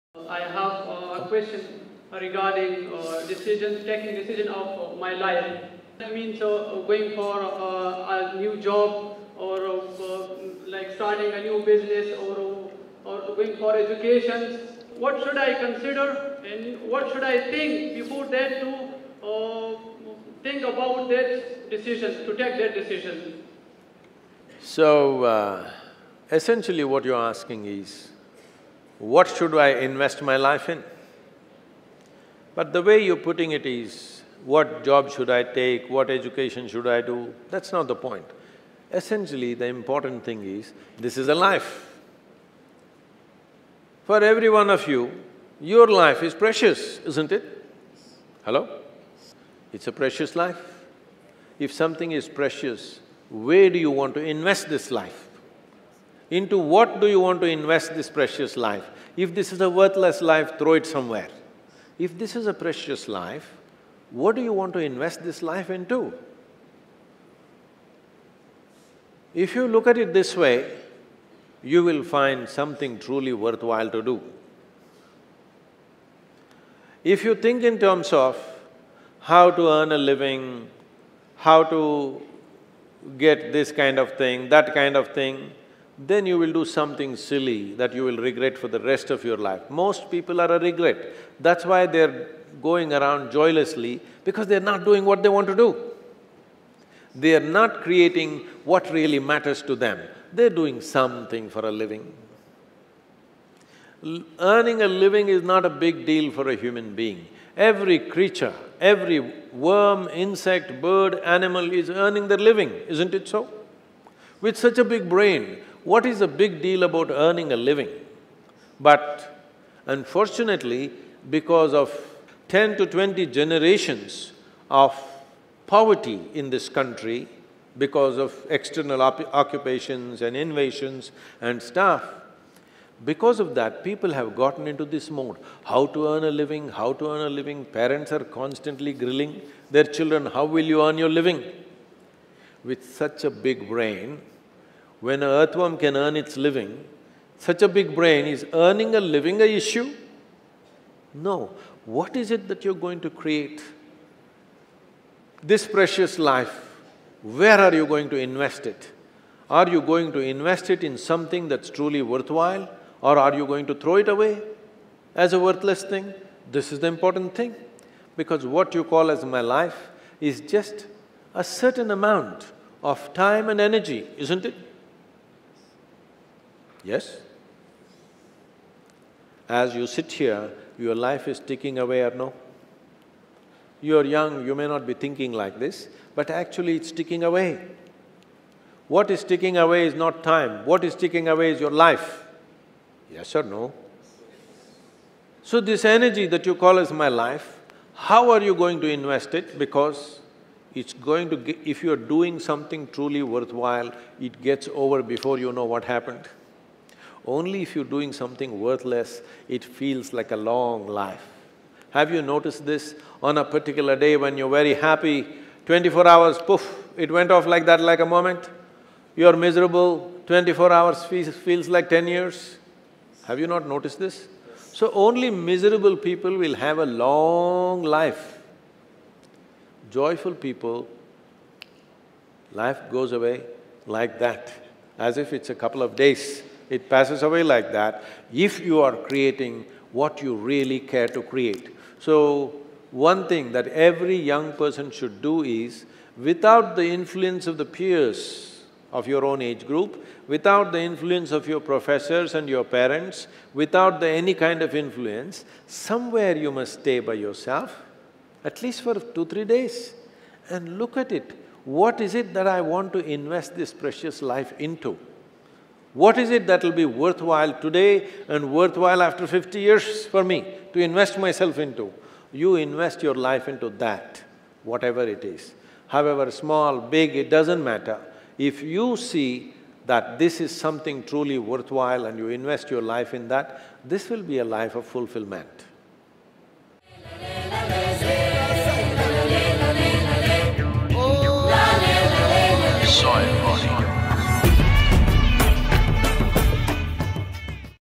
Come Prendere le Decisioni Importanti della Vita | Sadhguru Italiano
A cosa devo pensare prima di prendere le più importanti decisioni della mia vita?
Sadhguru risponde alla domanda di uno studente.